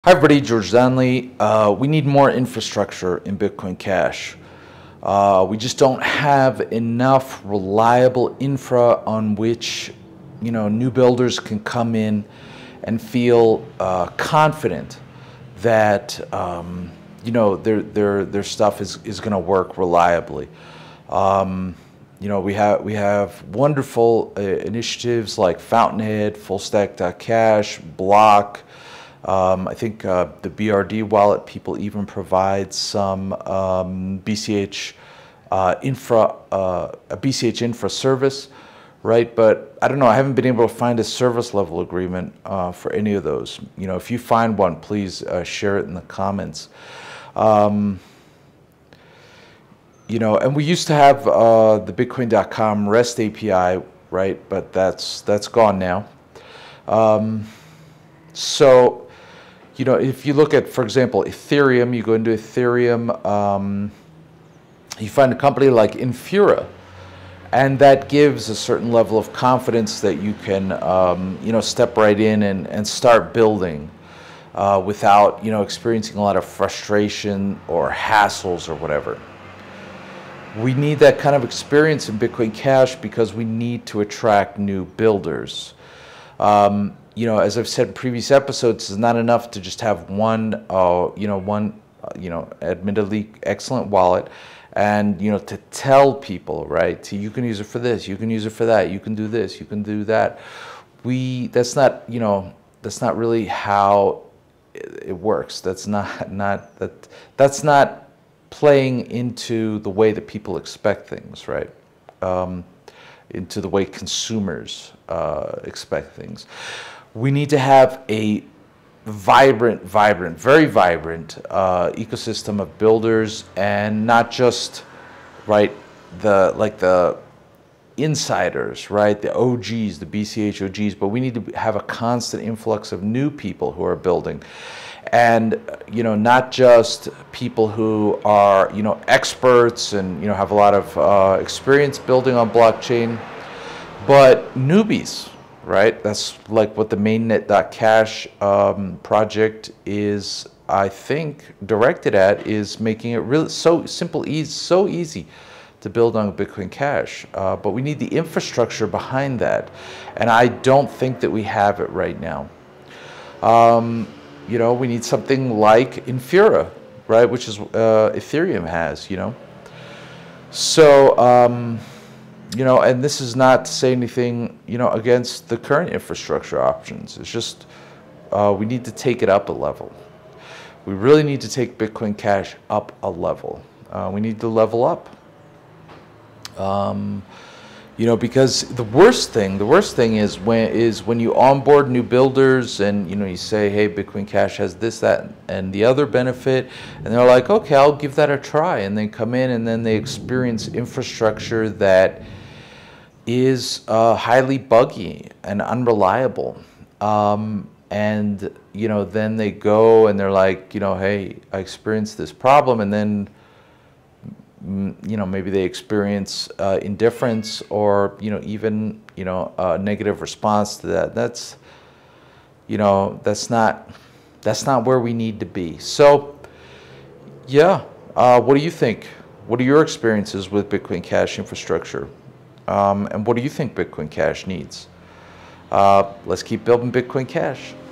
infra lacking
Infra Lacking: we lack infra in BCH, Bcom REST API, no SLA on other services. Hard for people to find a solid foundation to build on.
bitcoincash:qz3pxmwda8gd42wa8k9yfxcwhcaapeuhygjc8mc4m8